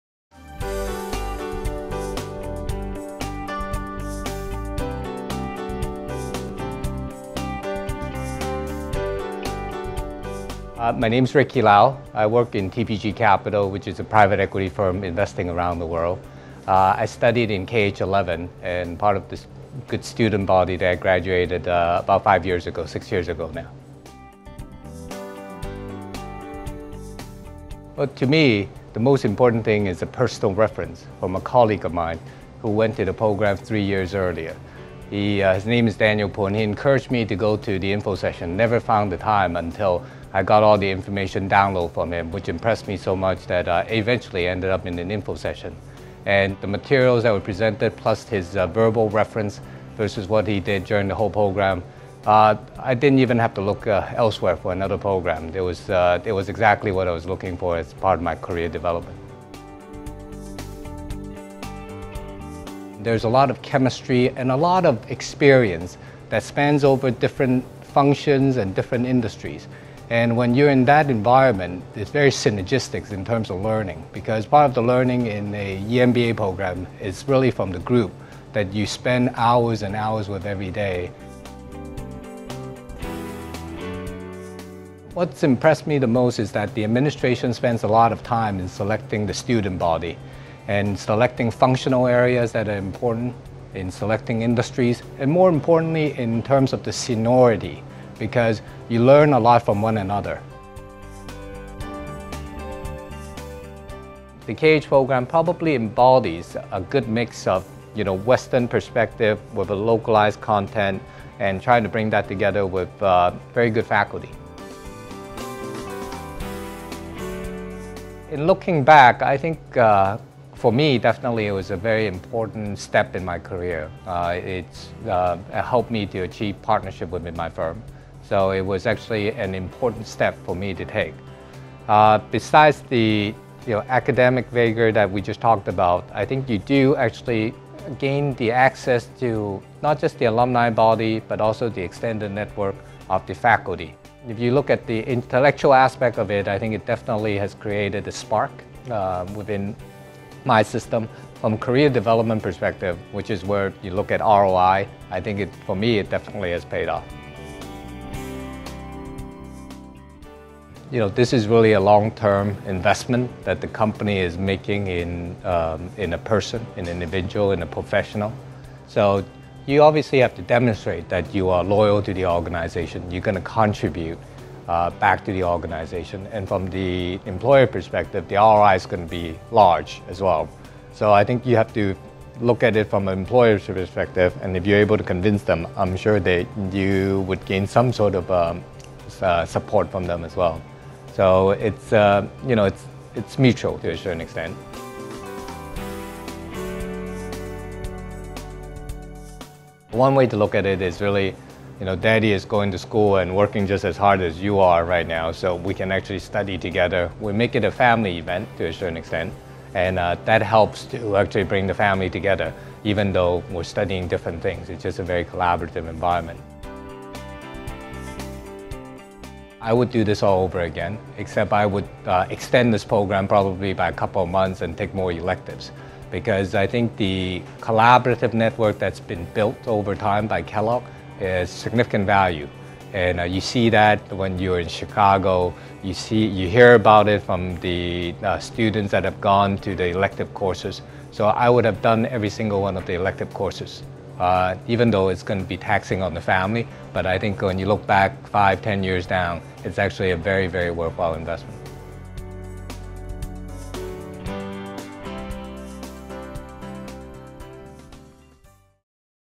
Need advice on how to seek company support for an EMBA?
Kellogg- HKUST Executive MBA Program

Ricky Lau (Class of 2009), Partner of TPG Capital Ltd., will tell you how he convinced the company to support his education plan!